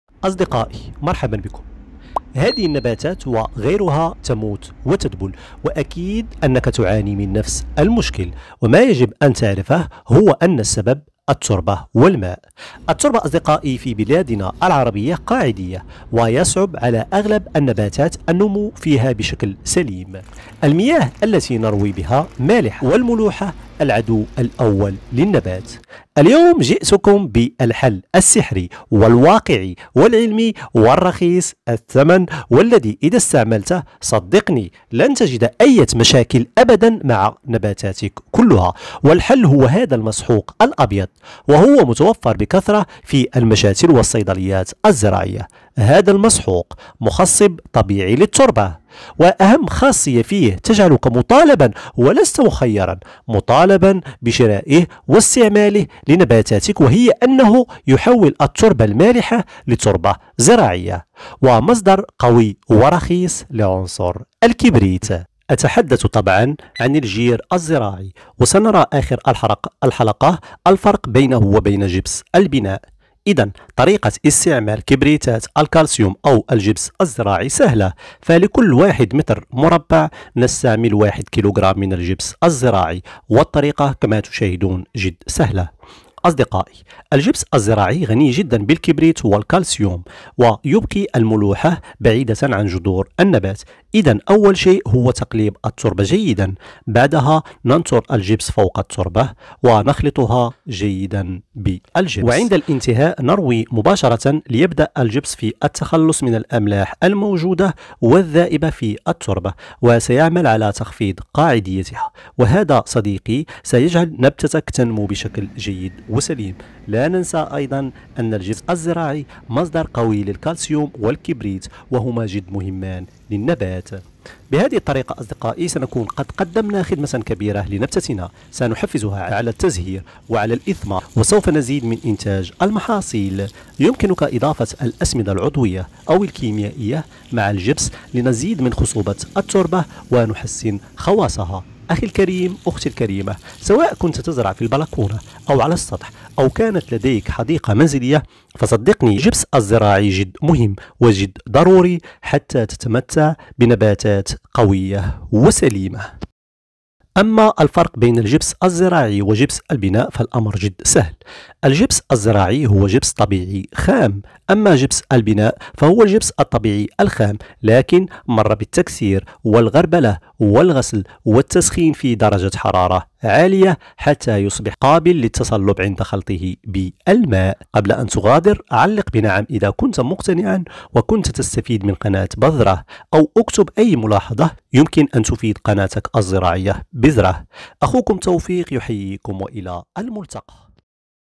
استعمل هذا السماد الطبيعي و تمتع نباتات قوية. و قل وداعا لمشاكل التربة الطينية و ملوحة ماء الري.
جبس الزراعي مخصب نقي طبيعي للتربة من كبريتات الكالسيوم المائية، وهو من أفضل الوسائل في علاج قلوية التربة والتخلص من الأملاح. The Role of Gypsum in Agriculture بذرة 2023 الفرق بين الجبس الزراعي و جبس البناء